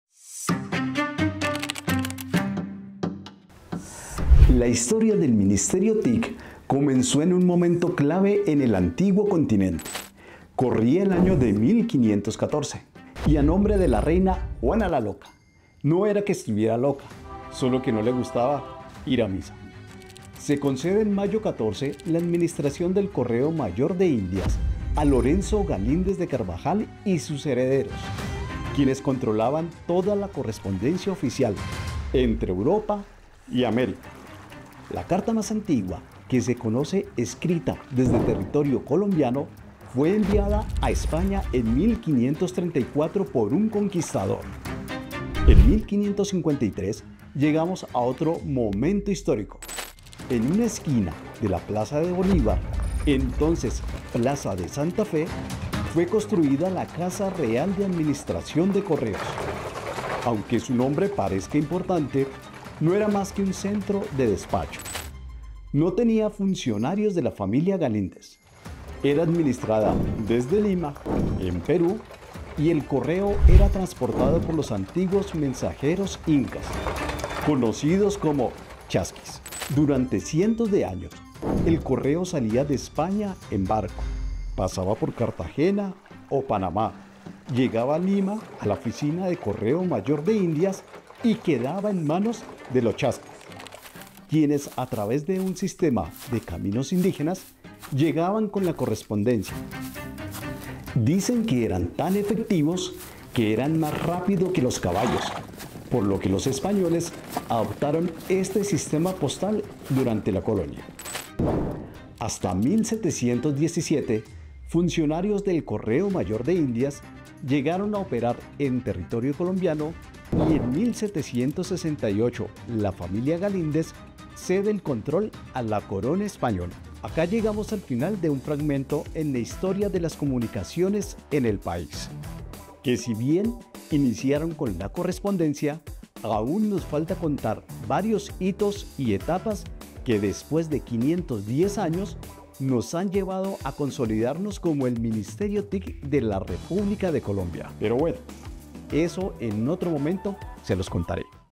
Así comienzan las comunicaciones en Colombia
Conéctate a la historia de las TIC en Colombia a través de los relatos e investigaciones de Néstor, nuestro historiador, el 'Correo Mayor de Indias' el precursor de la conexión intercontinental de Colombia con Europa, es allí donde se gesta por vez primera el uso de las TIC para la época como medio de intercambio de información.